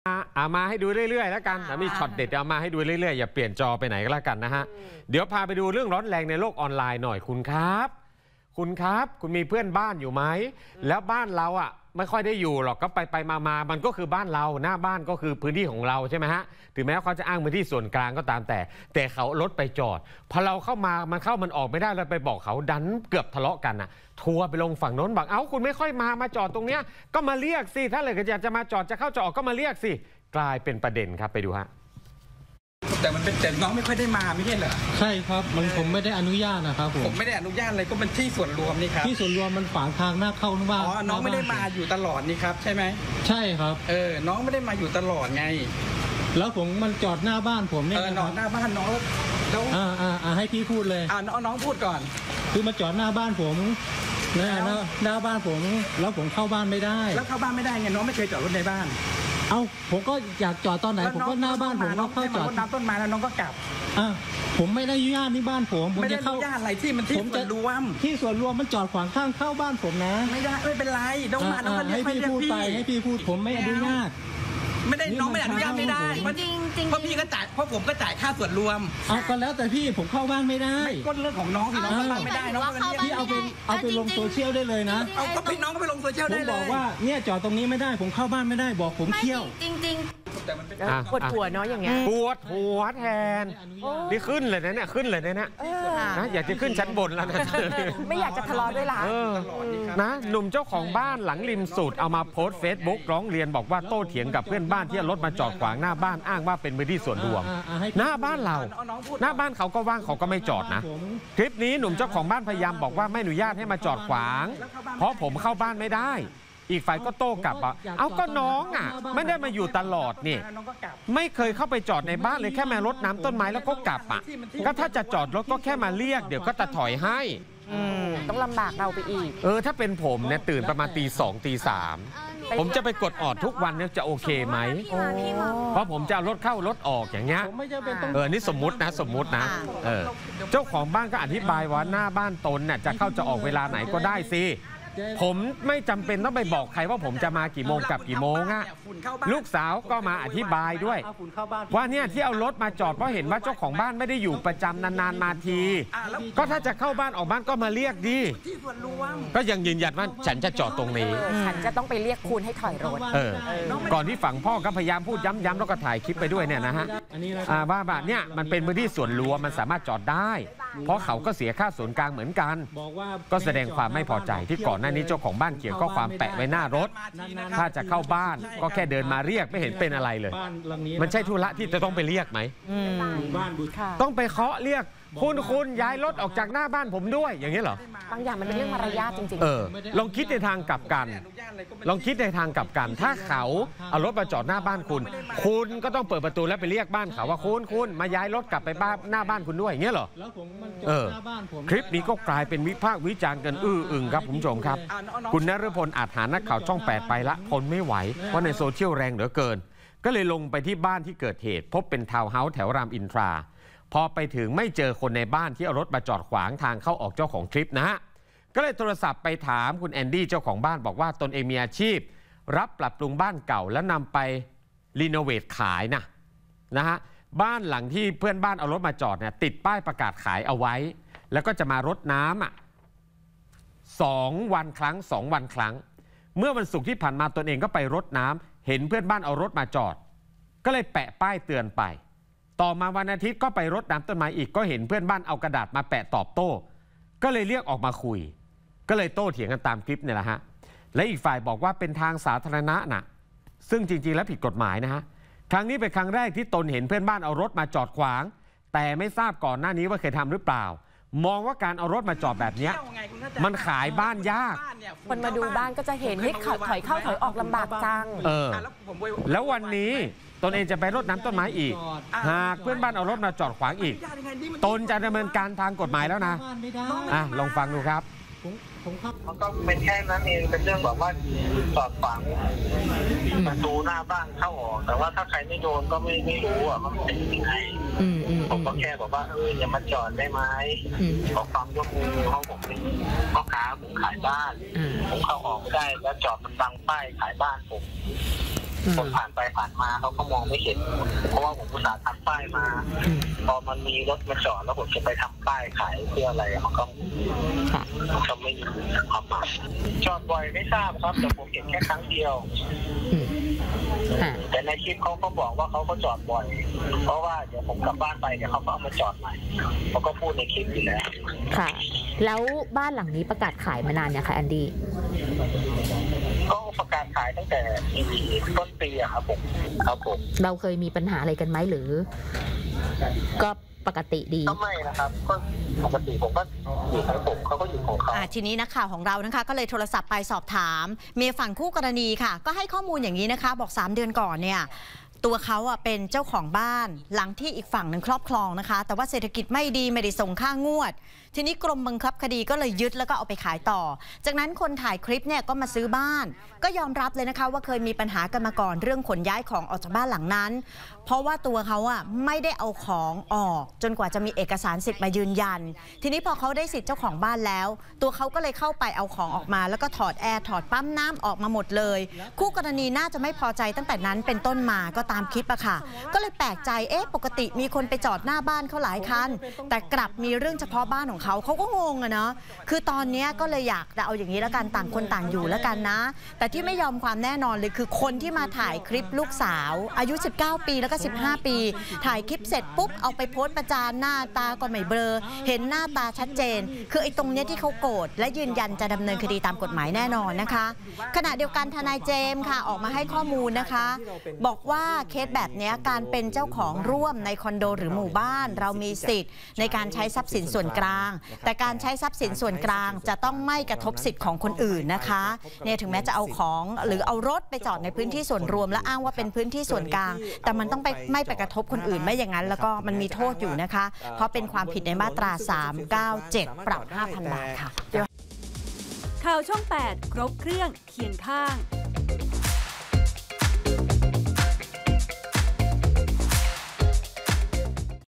หัวจะปวด เพื่อนบ้านดื้อจอดรถขวางบอกที่ส่วนรวม | ข่าวช่อง8
เพื่อนบ้าน จอดขวางทางเข้า อ้างสิทธิ์ เป็นพื้นที่ส่วนกลาง ทัวร์ลงกระหน่ำ